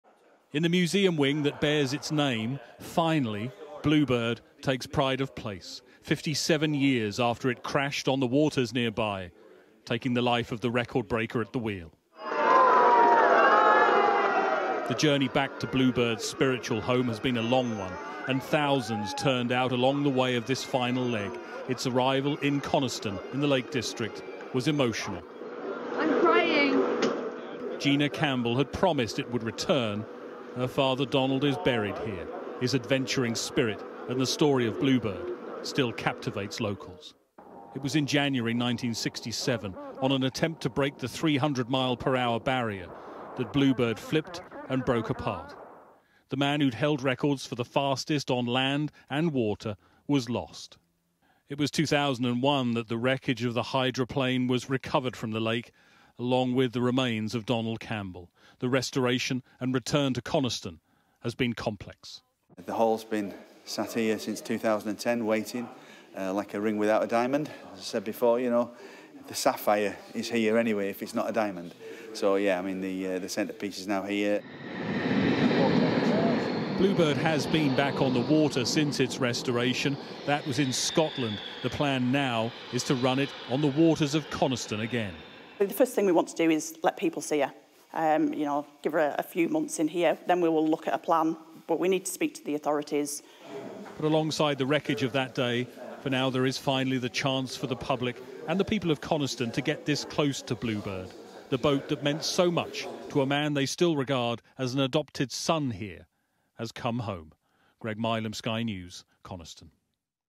Donald Campbell’s 'Bluebird' returns to Lake District’s Coniston
The "Bluebird" has returned to Coniston in the Lake District, 57 years after sinking in a fatal crash during a speed record attempt.

Donald Campbell died trying to reach 300mph in the hydroplane, which then spent many years at the bottom of Coniston Water.

Having been restored, its new home is now in the village museum.